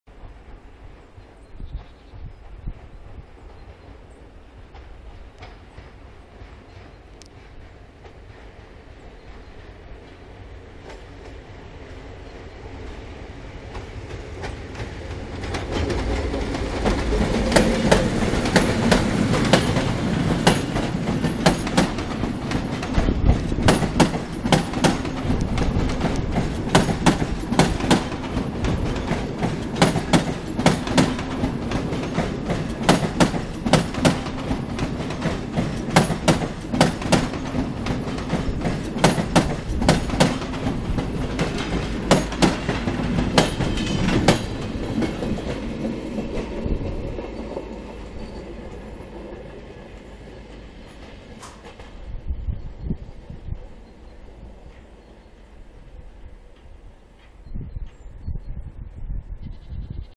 PENDOLINO dla PKP IC
kolejny skład dla IC - Pendolino nr 5 -
stacja Nędza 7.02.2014 g.11.33 skład przysypany śniegiem